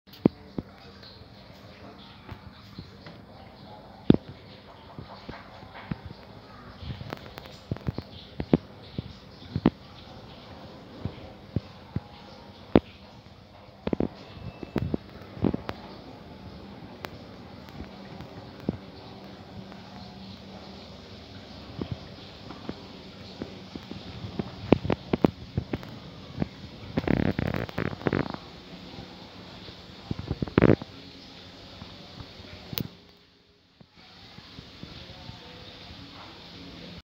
Gundam wing di atas aquarium
Bawah nya aquarium ikan mas koki taro gundam wing di atasnya keren juga😁👍